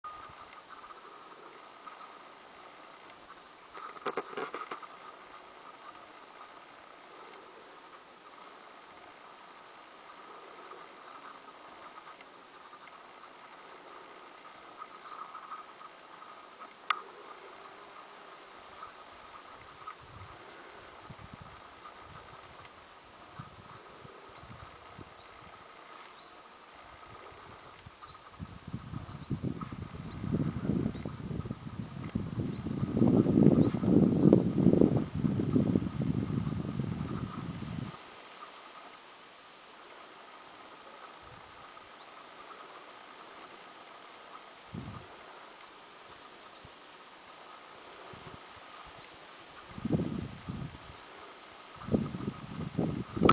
Rusałka Admirał
Rusałka Admirał widziana w Gorcach.